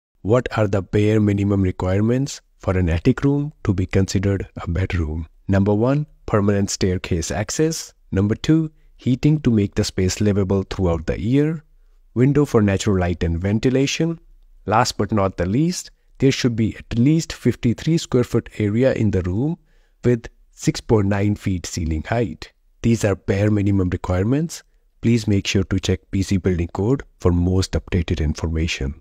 How can an attic meet bedroom requirements?
Attic room can be a bedroom?

Requirements are as following:

• Permanent staircase for access.

• Heating to make it livable throughout the year.

• A window for natural light and ventilation.

• At least 75 sq.ft without closet and 65 with closet, and 53 sq.ft area with at least 6.9 ft height.

If you want to learn more about how to do it, check out the BC building code for the most updated information.